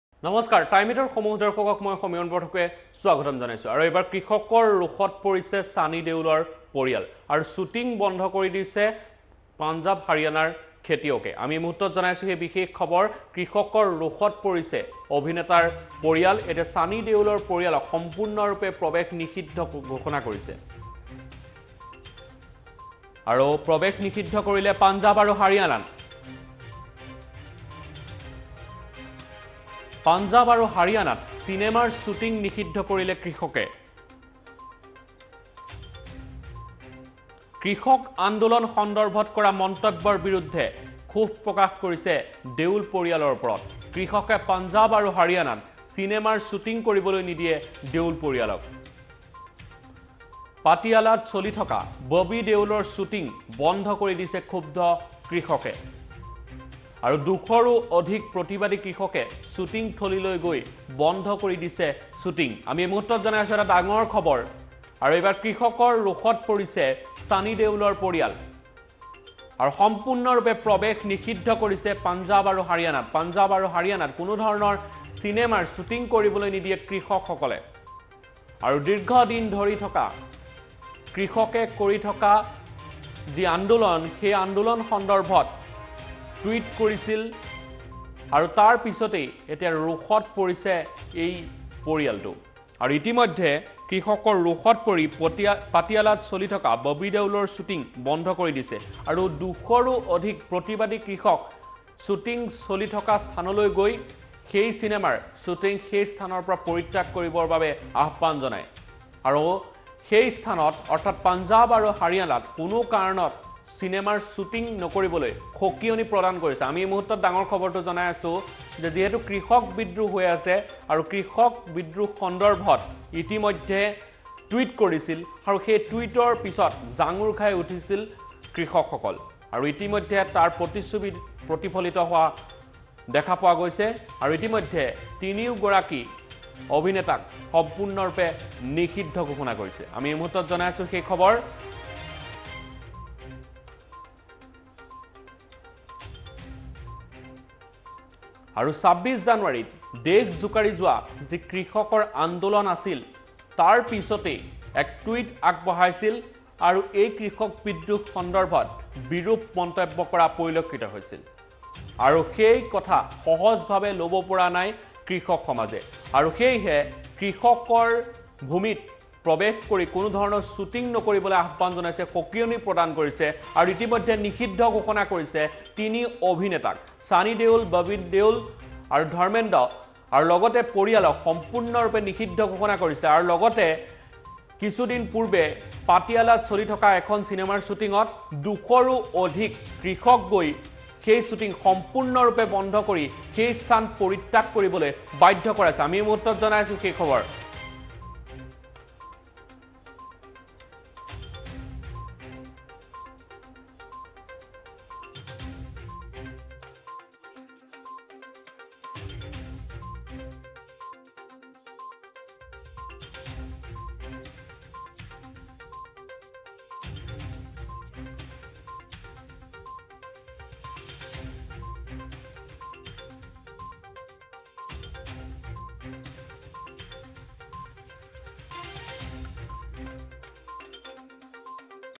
ছানী দেউলৰ পৰিয়ালক প্ৰৱেশত নিষিদ্ধ
প্ৰৱেশ নিষিদ্ধ কৰিলে পাঞ্জাৱ আৰু হাৰিয়ানাত
পাঞ্জাৱ আৰু হাৰিয়ানাত চিনেমাৰ শ্বুটিং নিষিদ্ধ কৰিলে কৃষকে
কৃষক আন্দোলন সন্দৰ্ভত কৰা মন্তব্যৰ বিৰুদ্ধে ক্ষোভ দেউল পৰিয়ালৰ ওপৰত
কৃষকে পাঞ্জাৱ আৰু হাৰিয়ানাত চিনেমাৰ শ্বুটিং কৰিবলৈ নিদিয়ে দেউল পৰিয়ালক
পাটিয়ালাত চলি থকা ববী দেউলৰ শ্বুটিং বন্ধ কৰি দিলে ক্ষুব্ধ কৃষকে
২০০ৰো অধিক প্ৰতিবাদী কৃষকে শ্বুটিং থলীলৈ গৈ বন্ধ কৰি দিয়ে শ্বুটিং